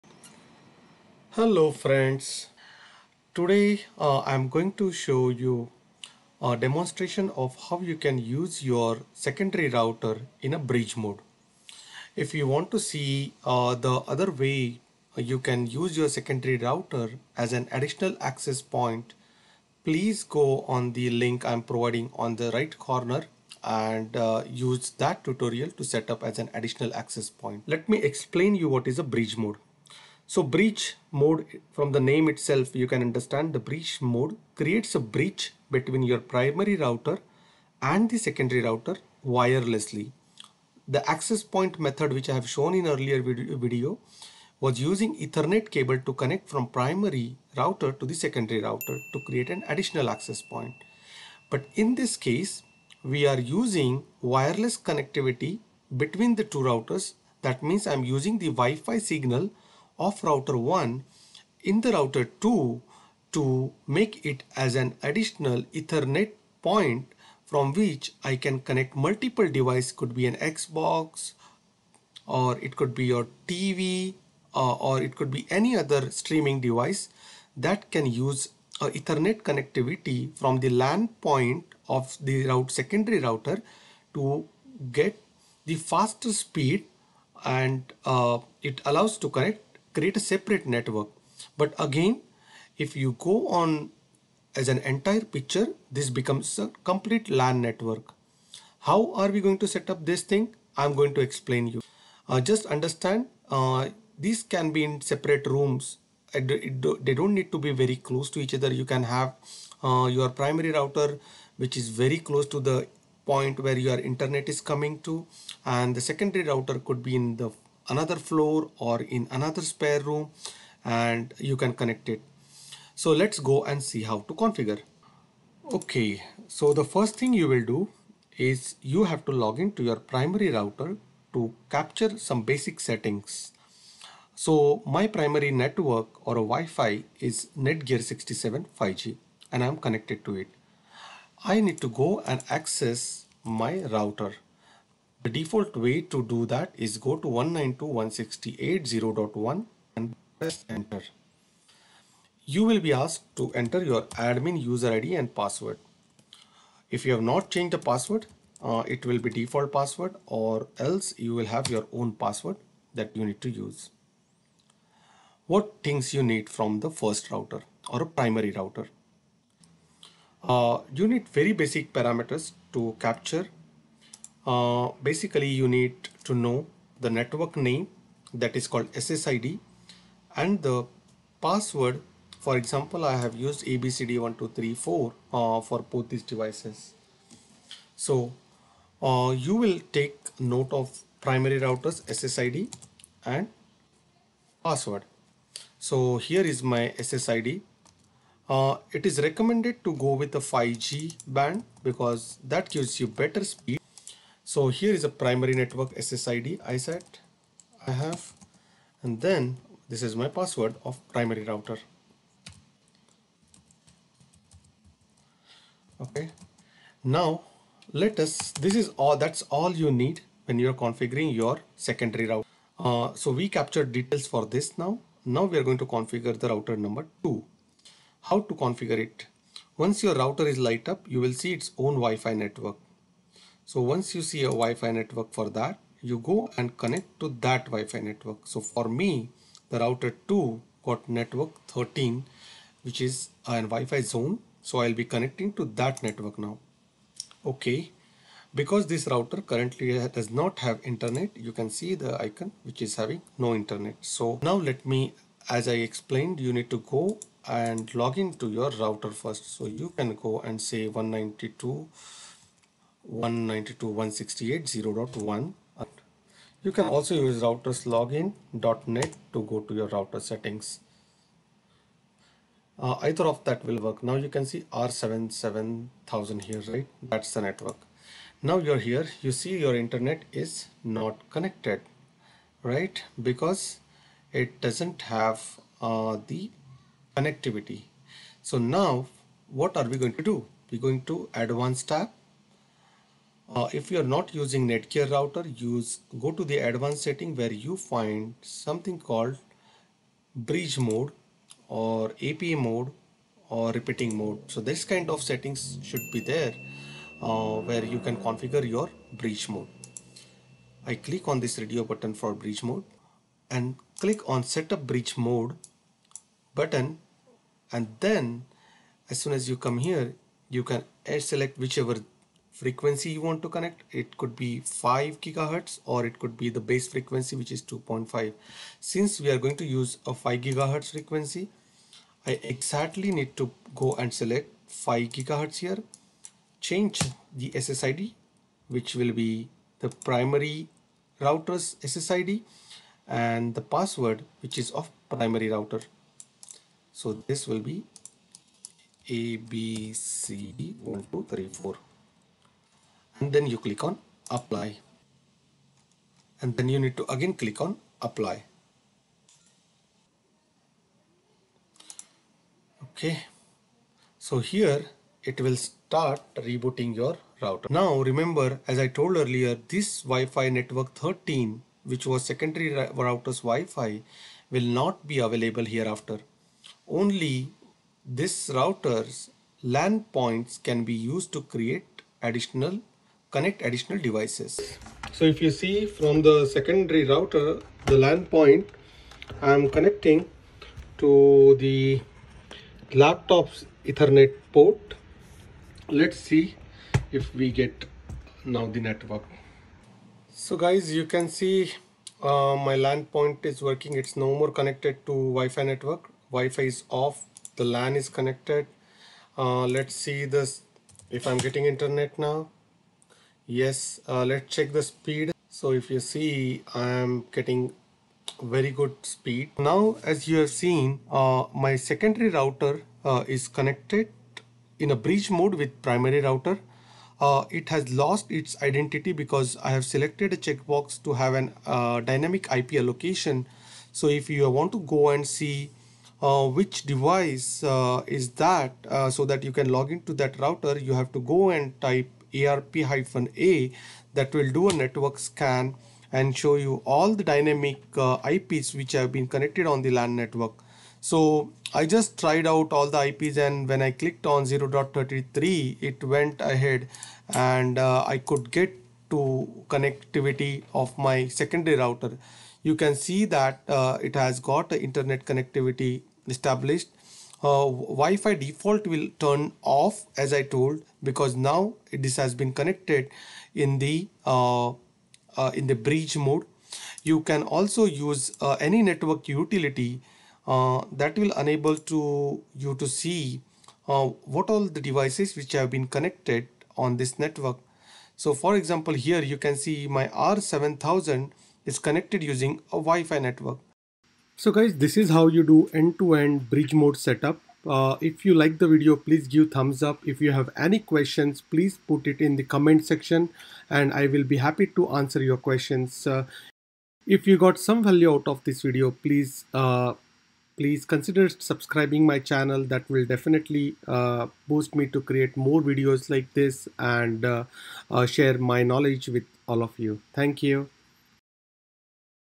Netgear Bridge Mode Setup | Wifi Extender | Secondary Router Setup
Watch this video to see how you can use your spare netgear router in bridge mode to connect with your primary router wirelessly.